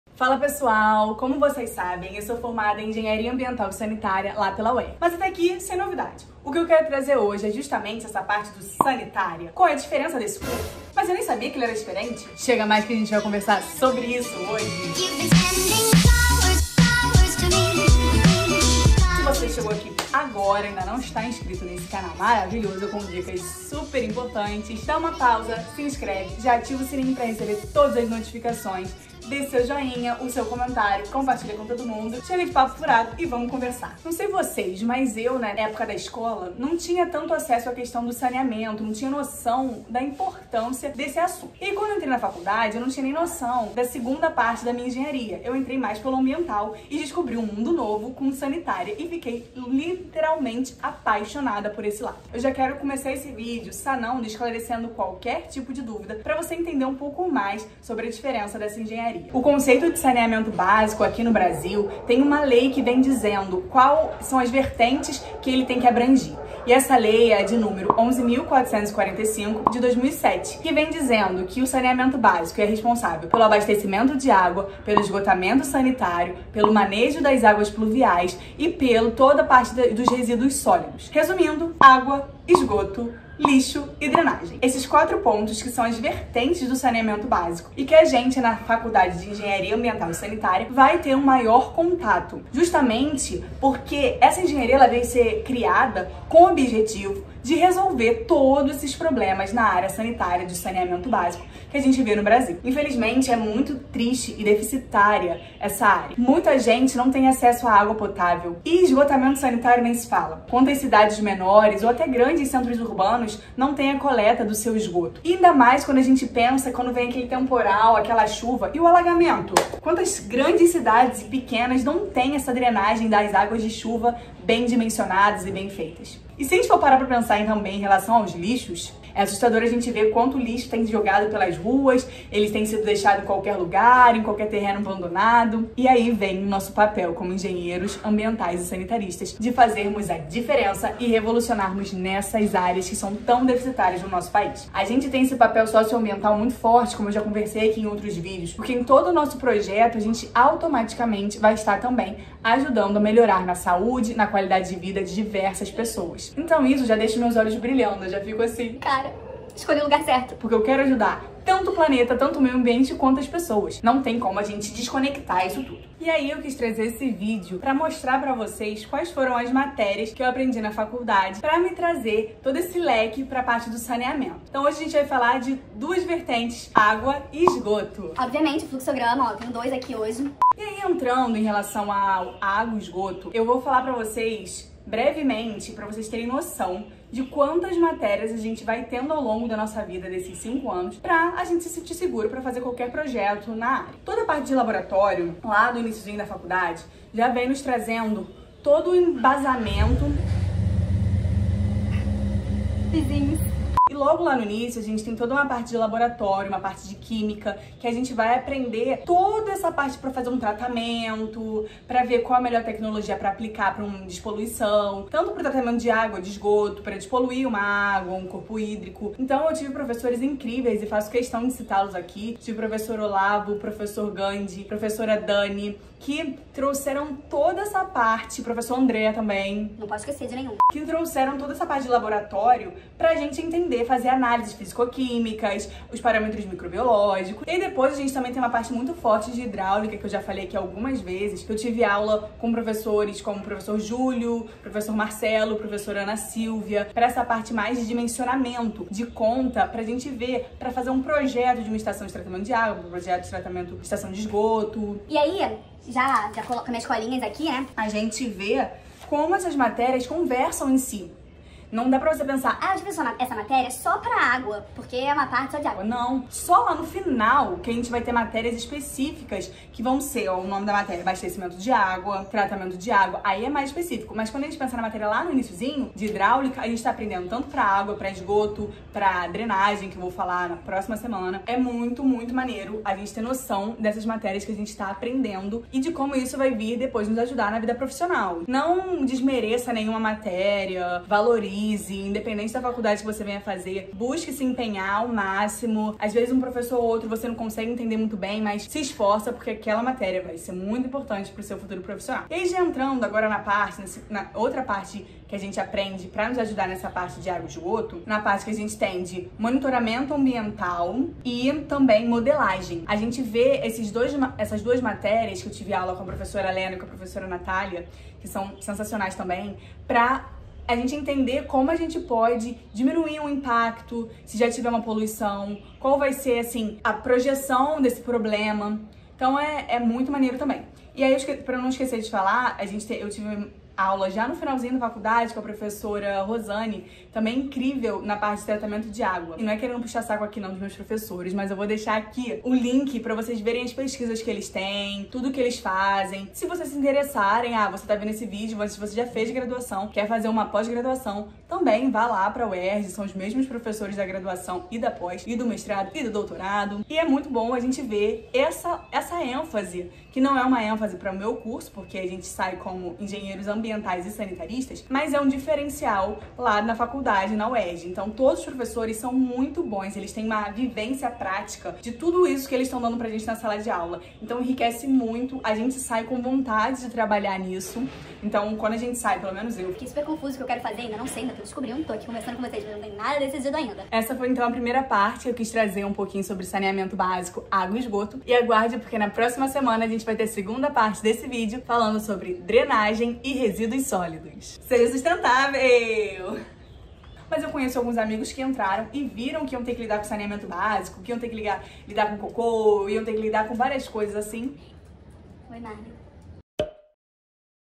QUAL É O DIFERENCIAL DA ENGENHARIA SANITÁRIA? | PARTE1 - ÁGUA E ESGOTO
Vem descobrir esse lado do saneamentobásico que a Eng Amb e Sanit (EAS) tem

Produção: Jornada Vídeos (@jornadavideos)
Roteiro: Rayssa Jacob
Edição: Bruno Imenes (@imbrunno)